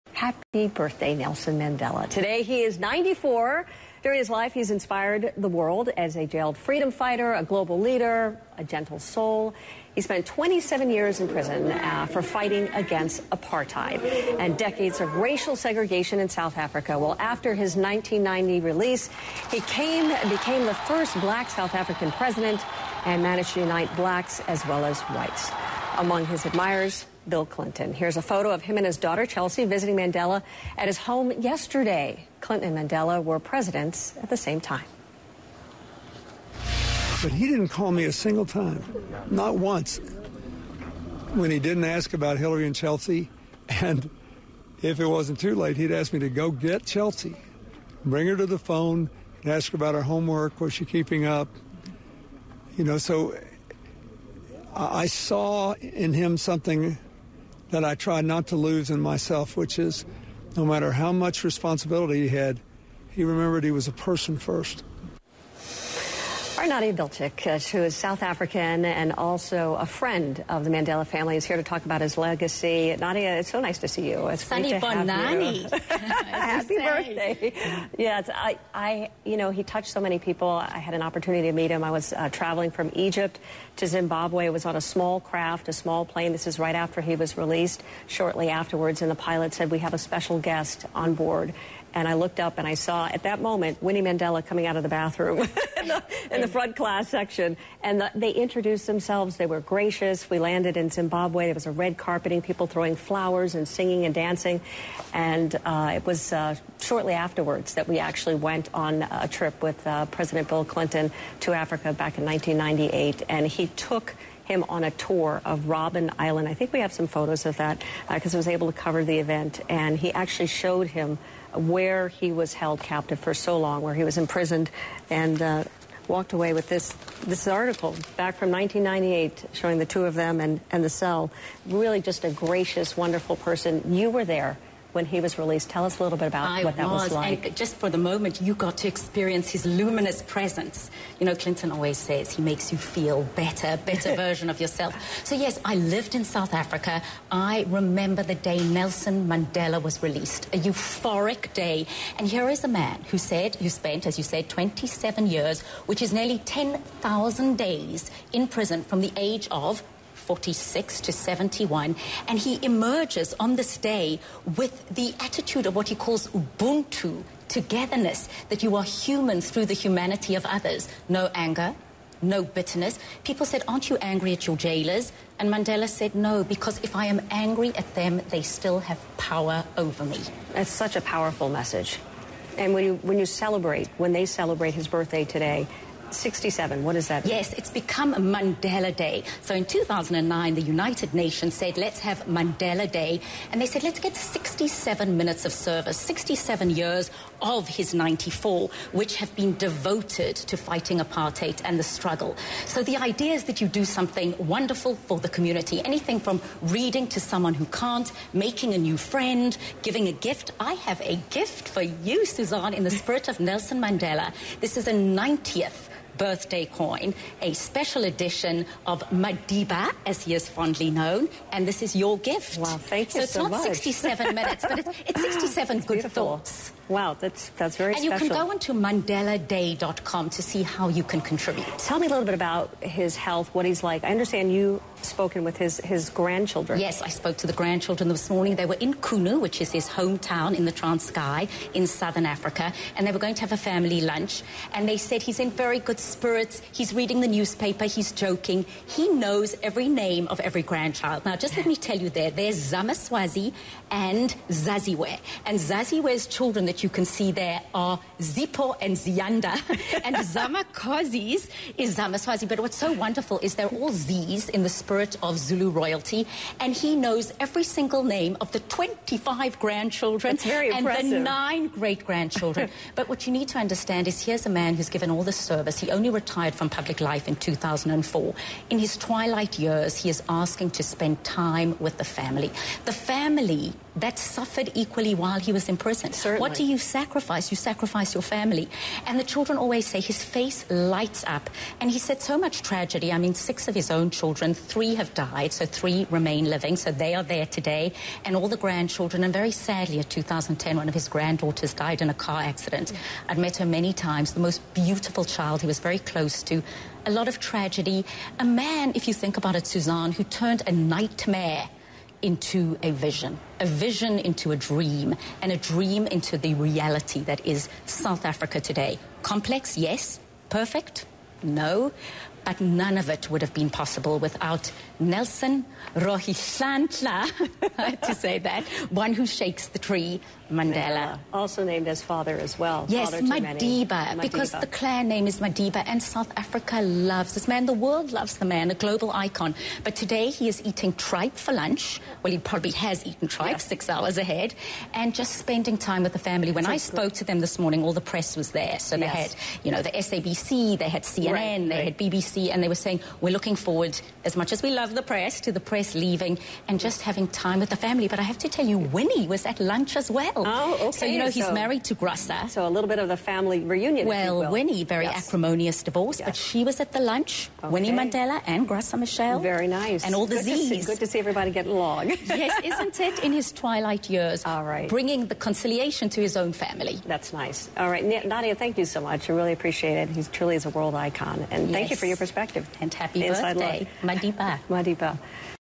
Mandela turns 94!
Nadia Bilchik talks to Suzanne Malveaux about Nelson Mandela's birthday and legacy around the world.